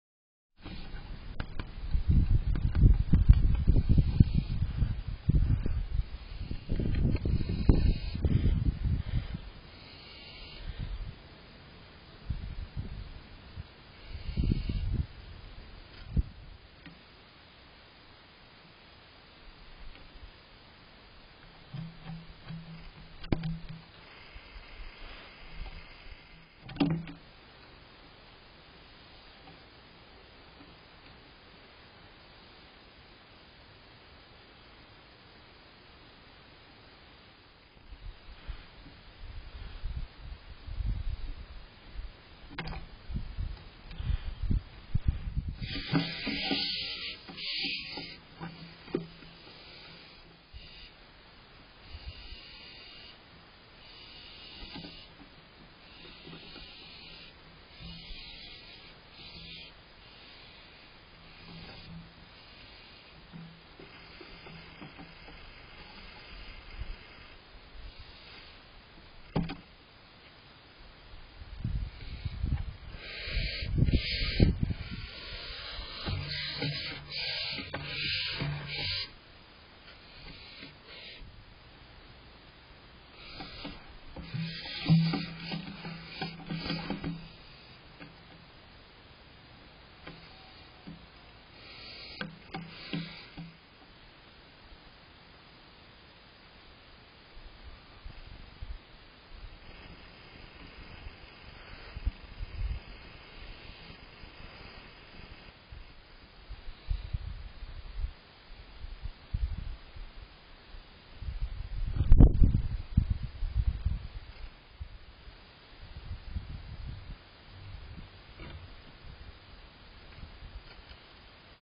Watch barn owl Owlet outside making first flying attempts whilst also collecting prey. Aug 24th
Older Owlet spending increasing amounts of time on platform during the night. Making attempts to fly short distances towards house. Gathers prey from parents and sharing them with sibling. @barnowlbob